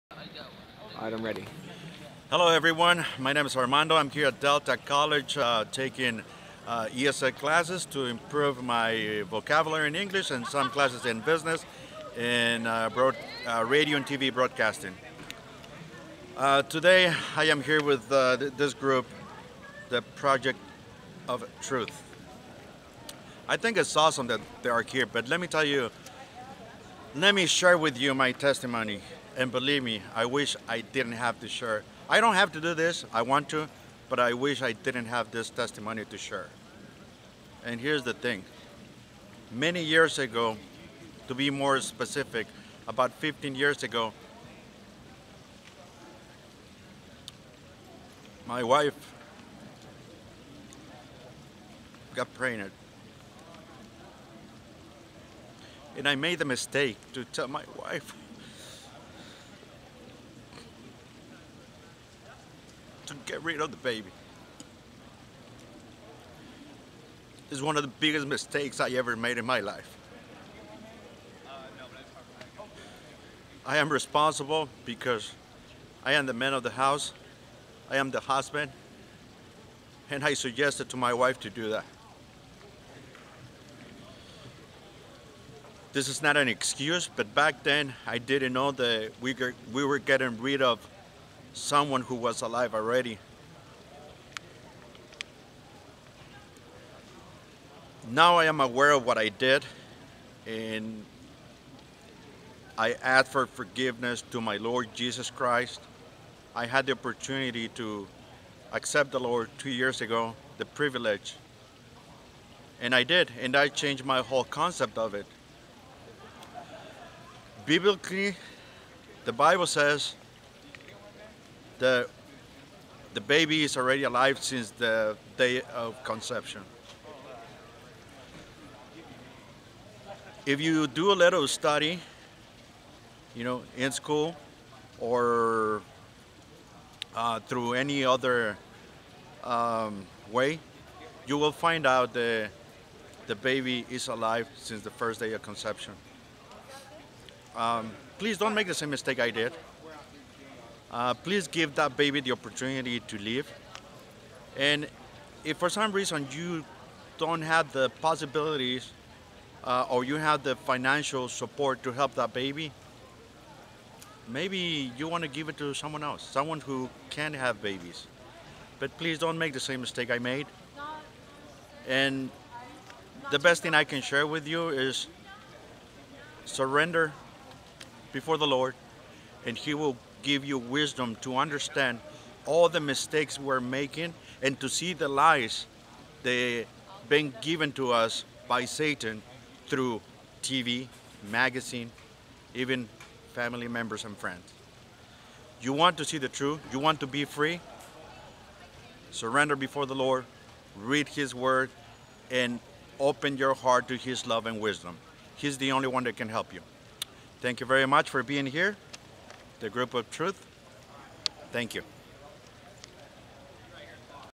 My Wife Got Pregnant - Armando
Armando gives his testimony of how Abortion had personally affected him.